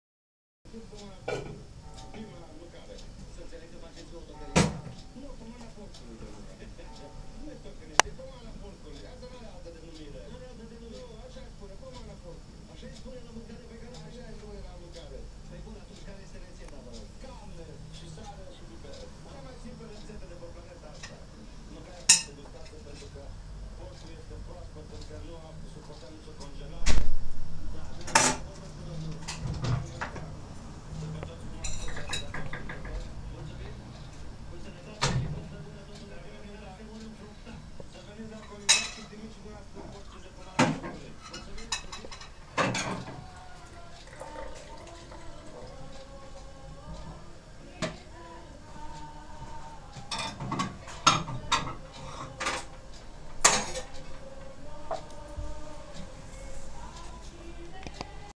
fumul de la centrala de apartament a vecinilor.avi
Polueaza sau nu polueaza centralele termice de apartament?
Fum se vede, miros are...Ce sa fie?
Zeci de ţevi de esapament pe blocuri...o minunatie!